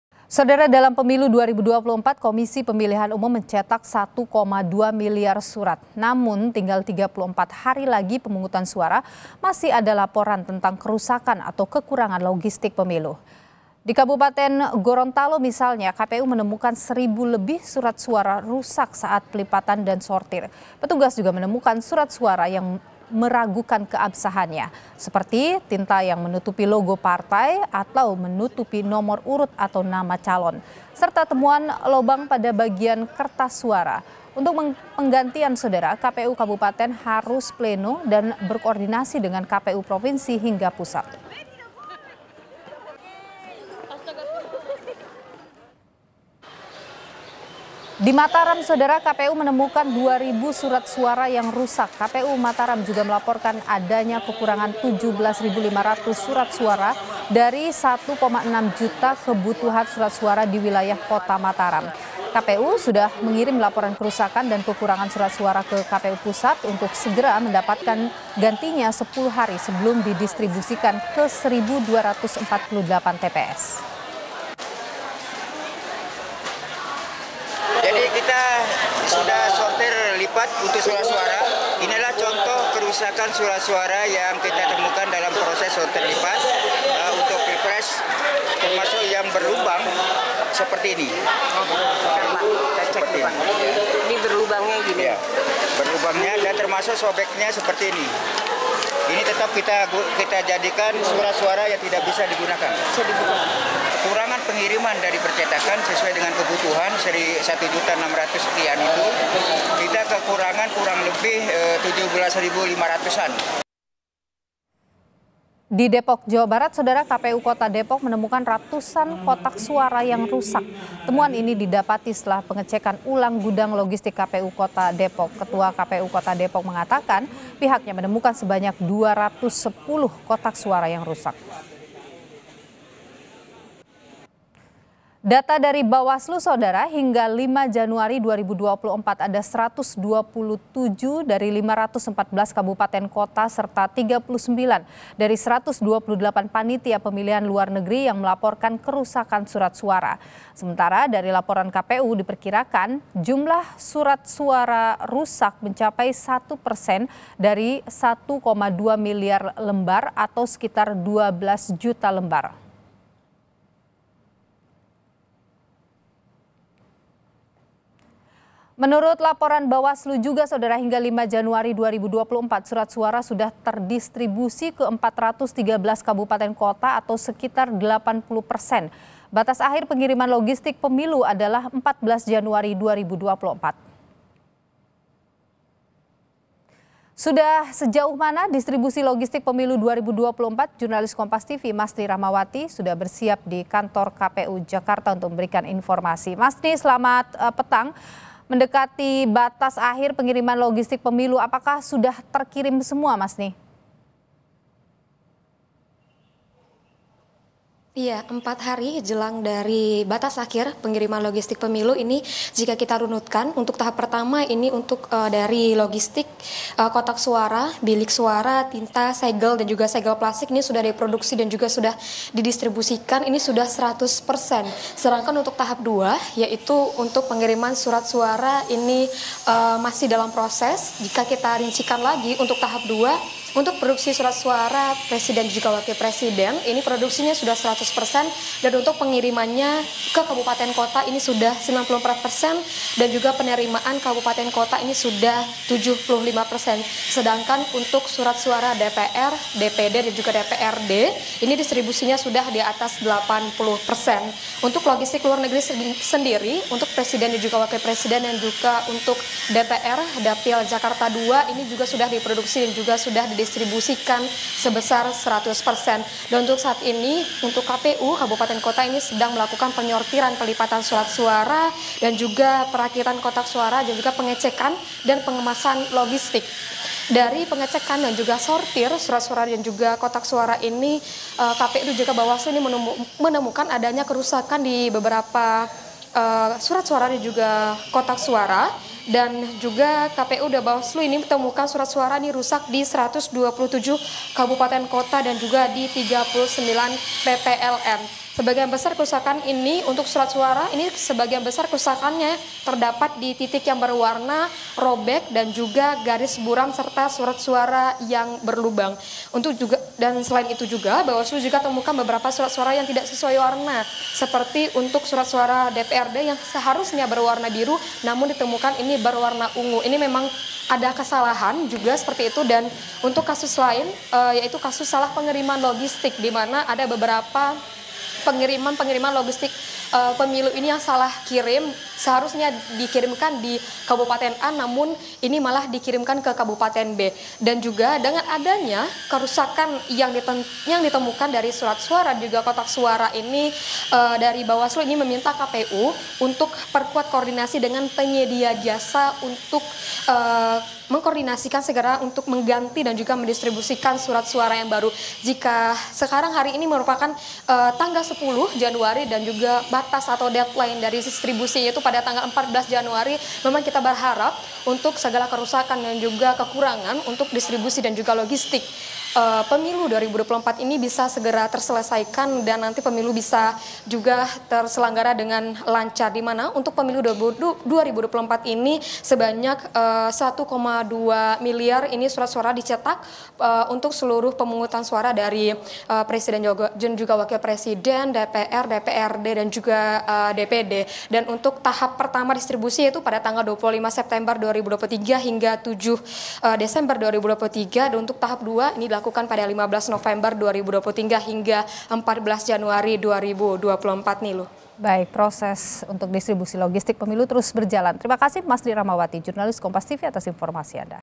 Jelang Pilpres 2024, KPU Temukan Ribuan Surat Suara Rusak & Masalah Logistik Pemilu
KOMPAS.TV - Dalam pemilu 2024, Komisi Pemilihan Umum mencetak 1,2 miliar surat. Namun tinggal 34 hari lagi pemungutan suara masih ada laporan tentang kerusakan atau kekurangan logistik pemilu.

Di Kabupaten Gorontalo misalnya, KPU menemukan 1.000 lebih surat suara rusak saat pelipatan dan sortir.

Petugas juga menemukan surat suara yang meragukan keabsahannya, seperti tinta yang  menutupi logo partai atau menutupi nomor urut atau nama calon, serta temuan lubang pada bagian kertas suara.

Untuk penggantian, KPU kabupaten harus pleno dan berkoordinasi dengan KPU propinsi hingga pusat.

Di Mataram, KPU menemukan 2.000 surat suara yang rusak. KPU Mataram juga melaporkan adanya kekurangan 17.500 surat suara dari 1,6 juta kebutuhan surat suara di wilayah Kota Mataram.

KPU sudah mengirim laporan kerusakan dan kekurangan surat suara  ke KPU Pusat untuk segera mendapatkan gantinya 10 hari sebelum didistribusikan ke 1.248 TPS.

Di Depok, Jawa Barat, KPU Kota Depok menemukan ratusan kotak suara yang rusak.

Temuan ini didapati setelah pengecekan ulang gudang logistik KPU Kota Depok.

Ketua KPU Kota Depok mengatakan pihaknya menemukan sebanyak 210 kotak suara yang rusak.